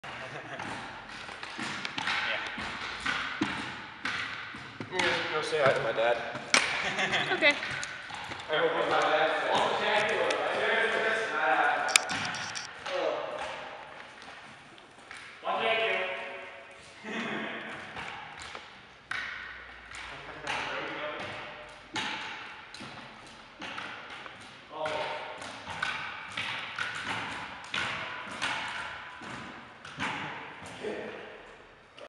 3 people first 45 minutes on jumping stilts at YMCA meet 12/29/11 (HD) (part 2 of 3)
3 of my friends trying out jumping stilts for the first time. They all did quite well. They were all on them for less than an hour.